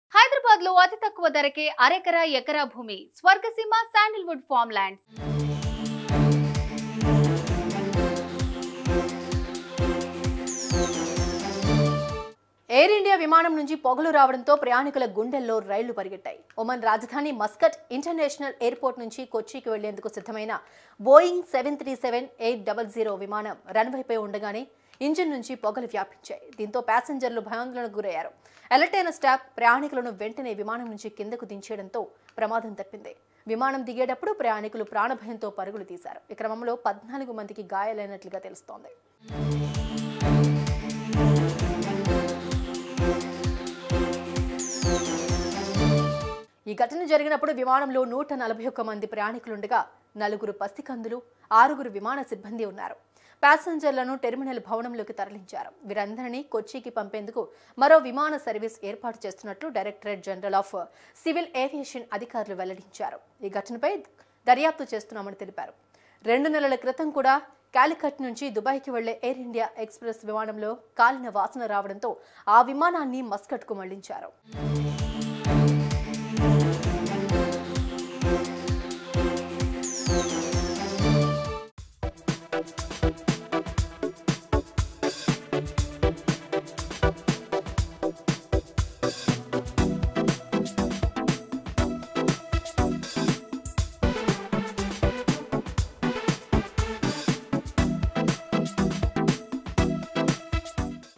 Air India Flight: Passengers Evacuated Air India Plane On Runway | @sumantvtelugulive​
Air India Flight: Passengers Evacuated Air India Plane On Runway |  @SumanTV Telugu ​